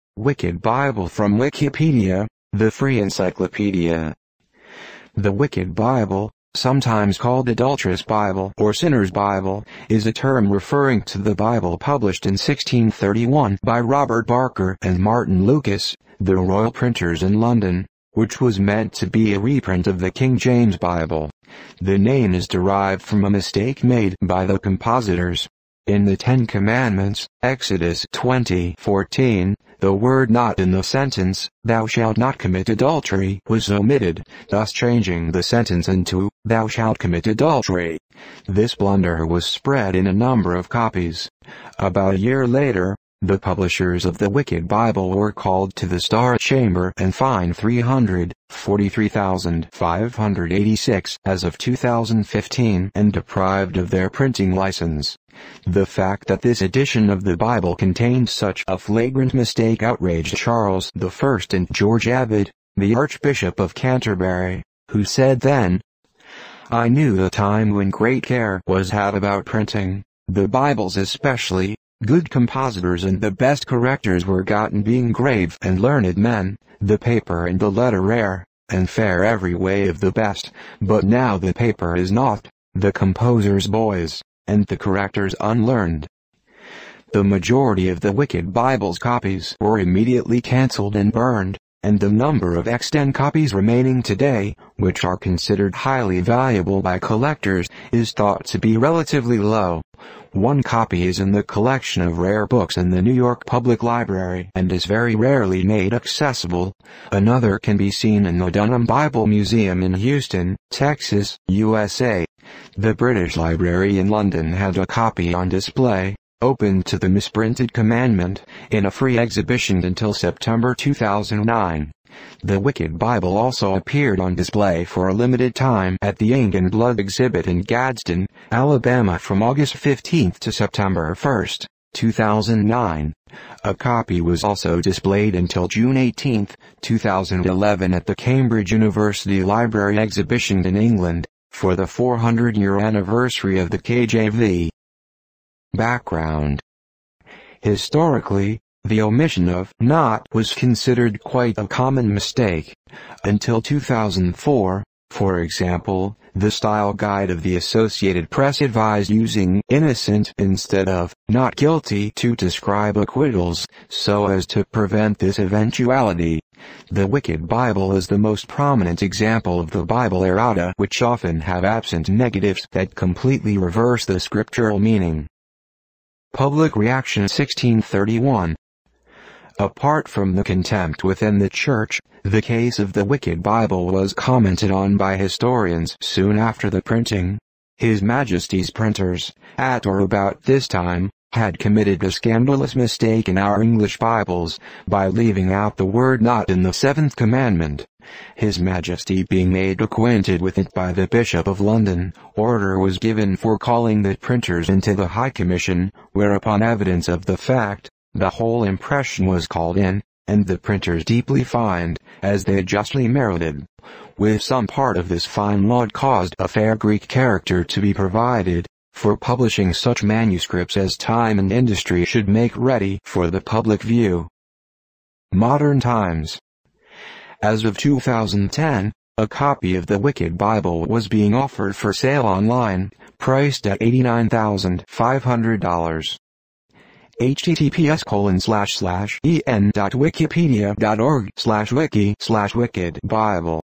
Wicked Bible or Sinners' Bible | From Wikipedia, the free encyclopedia | Audio Article
Audible Summer Promo
The Wicked Bible, sometimes called Adulterous Bible or Sinners' Bible, is a term referring to the Bible published in 1631 by Robert Barker and Martin Lucas, the royal printers in London, which was meant to be a reprint of the King James Bible. The name is derived from a mistake made by the compositors: in the Ten Commandments (Exodus 20:14), the word not in the sentence "Thou shalt not commit adultery" was omitted, thus changing the sentence into "Thou shalt commit adultery". This blunder was spread in a number of copies. About a year later, the publishers of the Wicked Bible were called to the Star Chamber and fined £300 (£43,586 as of 2015) and deprived of their printing license.The fact that this edition of the Bible contained such a flagrant mistake outraged Charles I and George Abbot, the Archbishop of Canterbury, who said then:

I knew the time when great care was had about printing, the Bibles especially, good compositors and the best correctors were gotten being grave and learned men, the paper and the letter rare, and faire every way of the best, but now the paper is nought, the composers boys, and the correctors unlearned.

The majority of the Wicked Bible's copies were immediately cancelled and burned, and the number of extant copies remaining today, which are considered highly valuable by collectors, is thought to be relatively low. One copy is in the collection of rare books in the New York Public Library and is very rarely made accessible; another can be seen in the Dunham Bible Museum in Houston, Texas, USA. The British Library in London had a copy on display, opened to the misprinted commandment, in a free exhibition until September 2009. The Wicked Bible also appeared on display for a limited time at the Ink and Blood Exhibit in Gadsden, Alabama from August 15 to September 1, 2009. A copy was also displayed until June 18, 2011 at the Cambridge University Library exhibition in England, for the 400-year anniversary of the KJV.
Background
Historically, the omission of "not" was considered quite a common mistake. Until 2004, for example, the style guide of the Associated Press advised using "innocent" instead of "not guilty" to describe acquittals, so as to prevent this eventuality. The Wicked Bible is the most prominent example of the bible errata which often have absent negatives that completely reverse the scriptural meaning.
Public reaction
1631
Apart from the contempt within the church, the case of the Wicked Bible was commented on by historians soon after the printing:
His Majesties Printers, at or about this time, had committed a scandalous mistake in our English Bibles, by leaving out the word Not in the Seventh Commandment. His Majesty being made acquainted with it by the Bishop of London, Order was given for calling the Printers into the High-Commission, whereupon Evidence of the Fact, the whole Impression was called in, and the Printers deeply fined, as they justly merited. With some part of this Fine Laud caused a fair Greek Character to be provided, for publishing such Manuscripts as Time and Industry should make ready for the Public view.
Modern times
As of 2010, a copy of the Wicked Bible was being offered for sale online, priced at $89,500.